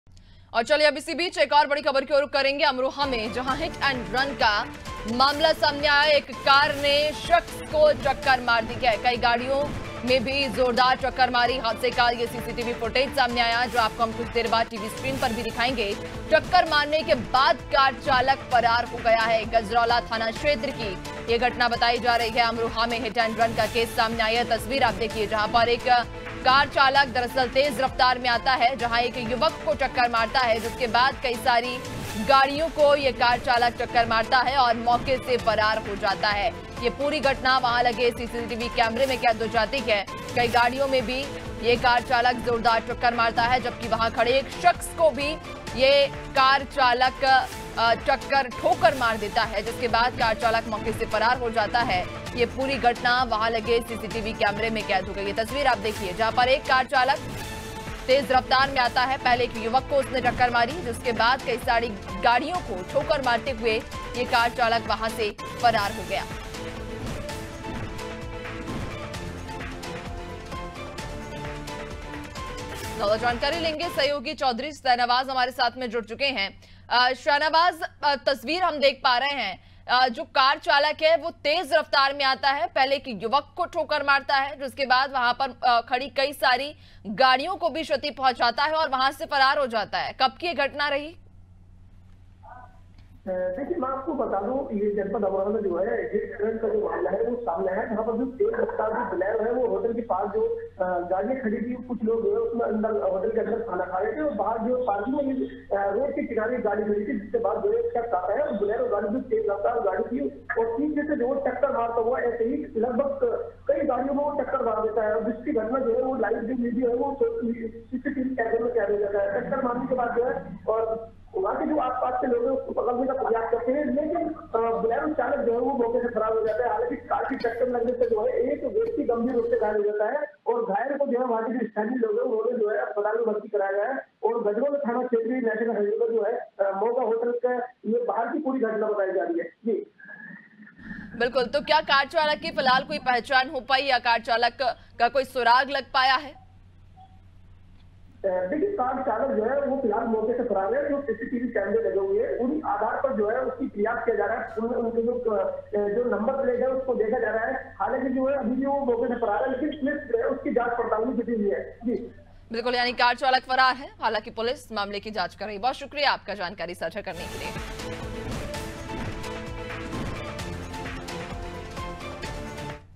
Amroha : अमरोहा में हिट एंड रन, कार ने एक शख्स को मारी टक्कर || Anaadi Tv
अमरोहा में हिट एंड रन
कार ने एक शख्स को मारी टक्कर
कई गाड़ियों में भी मारी जोरदार टक्कर
हादसे का CCTV फुटेज आया सामने
टक्कर मारने के बाद कार चालक फरार
गजरौला थाना क्षेत्र की घटना

HATHWAY- 231
SITI- 356
SKY- 441
ACN- 231
AIRTEL - 379
JIO TV, MX PLAYER, VI